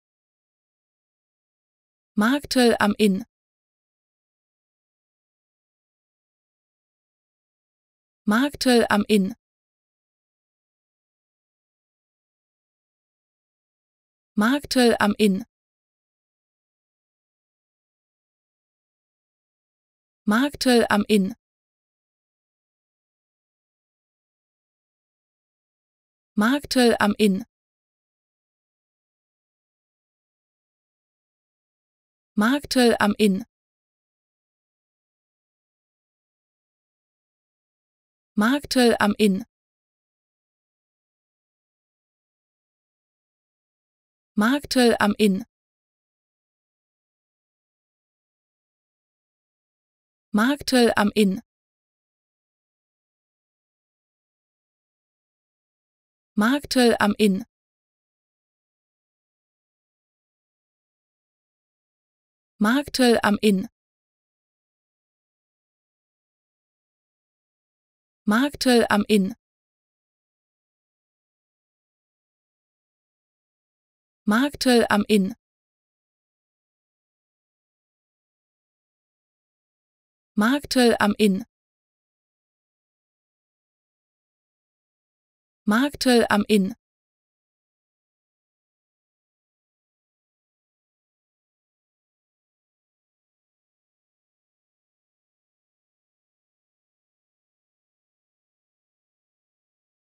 How to Pronounce ''Marktl am Inn'' Correctly in German
Learn how to say ''Marktl am Inn'' in German with this short pronunciation tutorial.